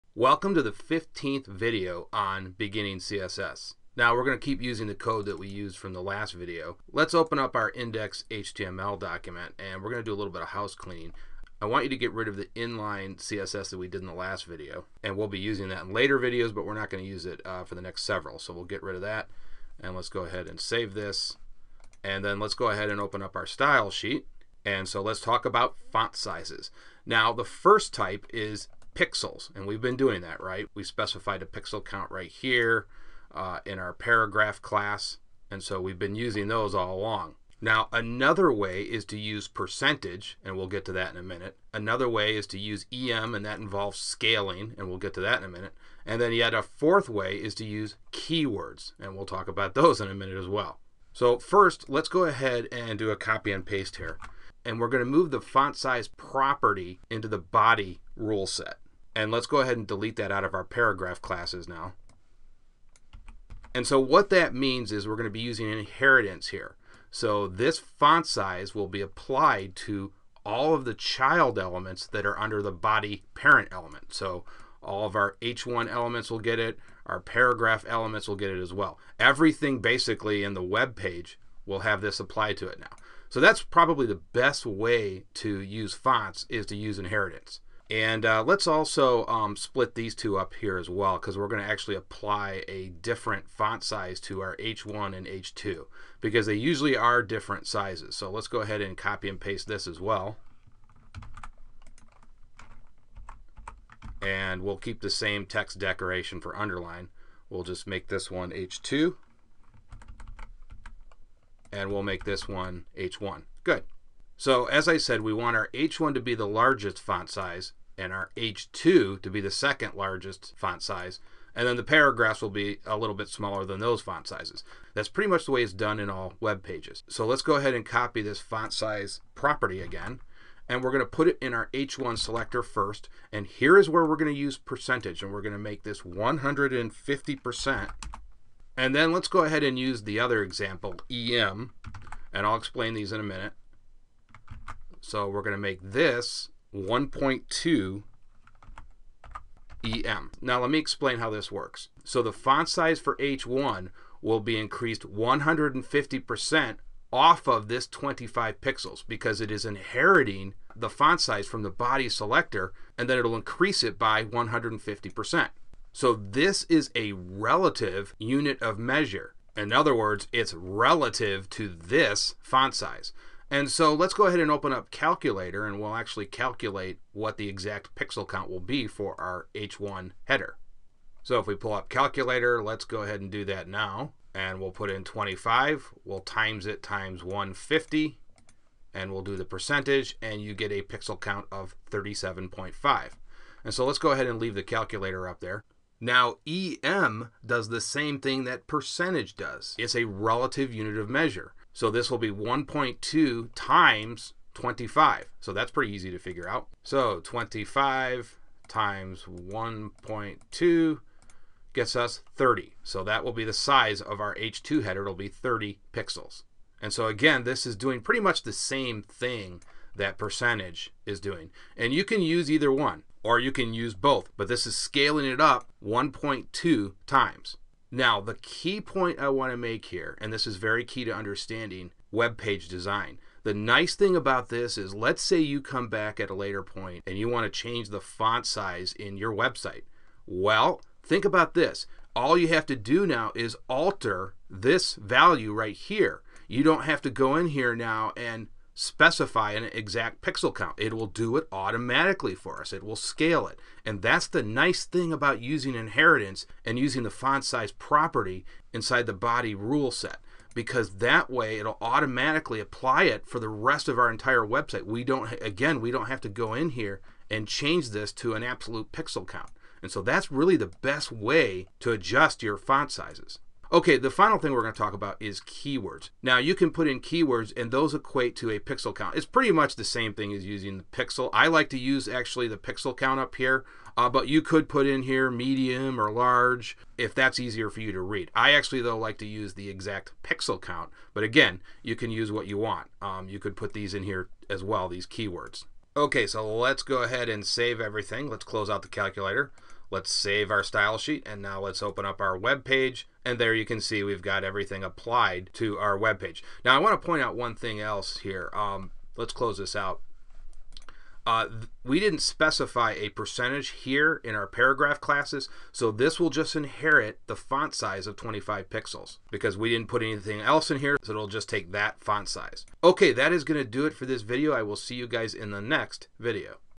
CSS Tutorial for Beginners - 15 - More on Font sizes
In this video we take a longer look at font sizes.